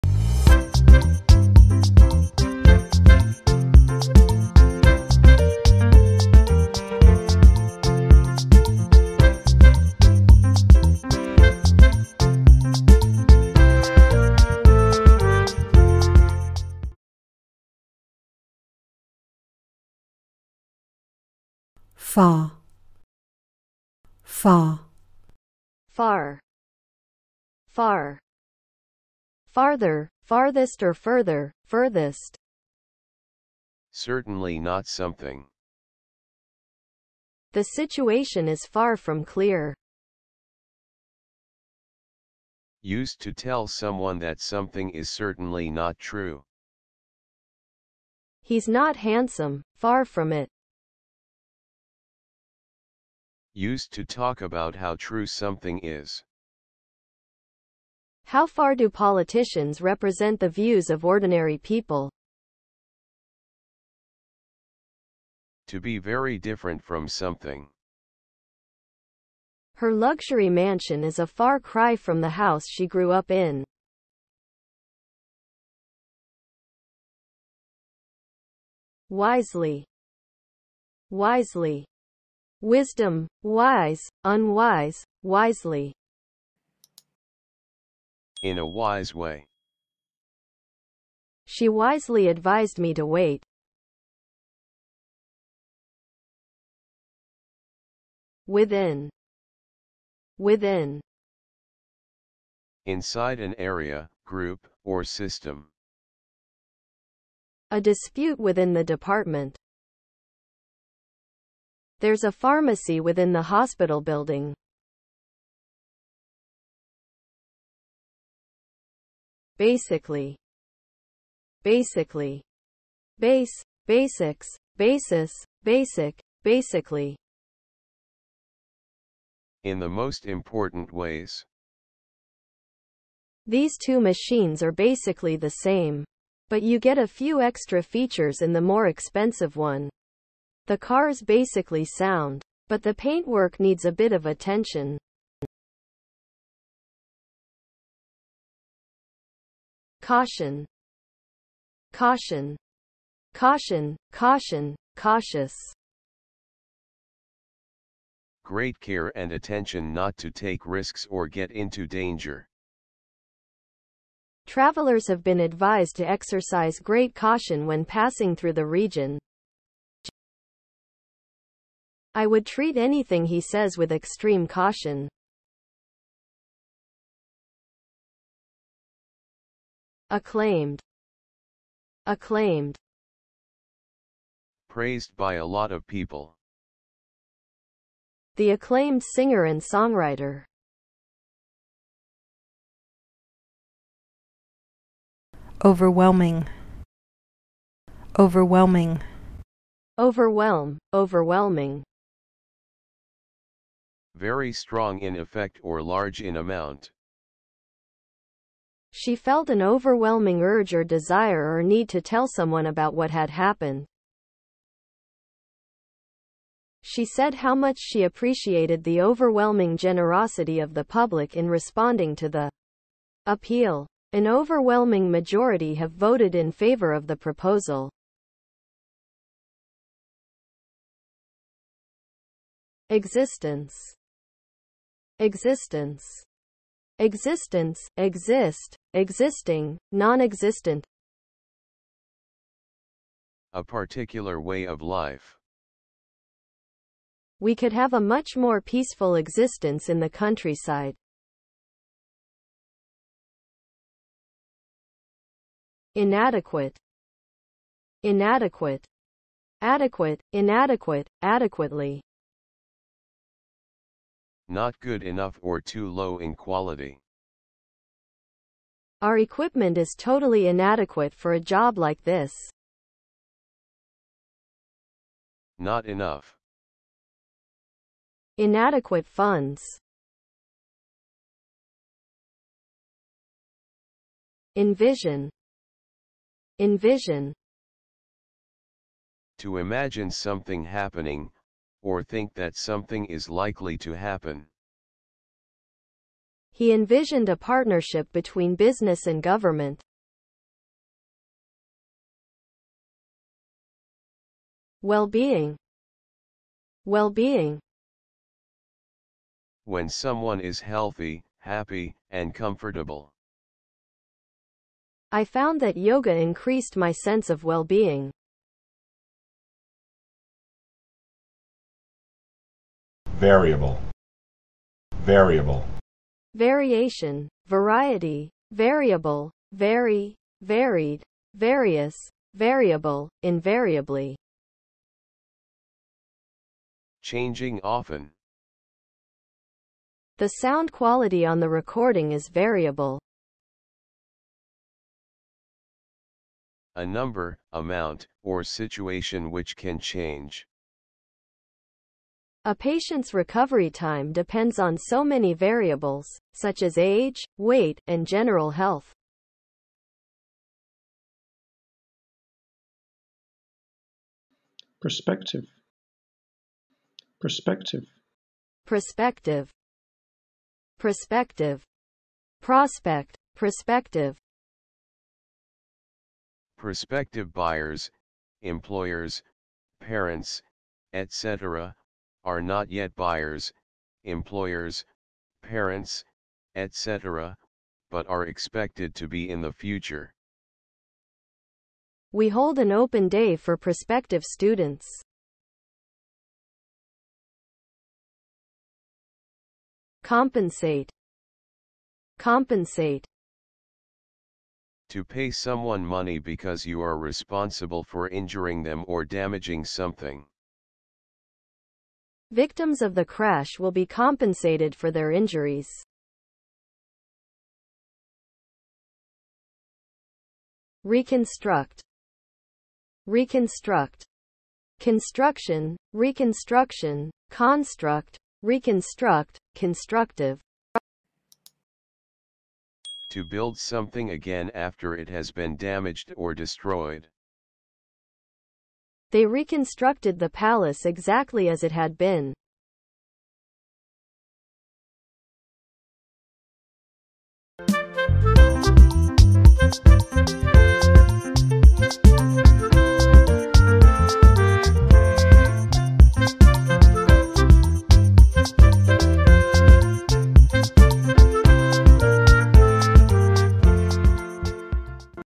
English Vocabulary Simplified: C1 Level for Advanced Learners #46
★ TABLE OF CHAPTERS IN THIS VIDEO ★
• 00:00 Introduction
• 00:21 far
• 01:18 wisely
• 01:37 within
• 01:58 basically
• 02:29 caution
• 02:59 acclaimed
• 03:14 overwhelming
• 03:52 existence
• 04:15 inadequate
• 04:43 envision
• 05:03 well-being
• 05:22 variable
• 06:03 prospective
• 06:39 compensate
• 07:00 reconstruct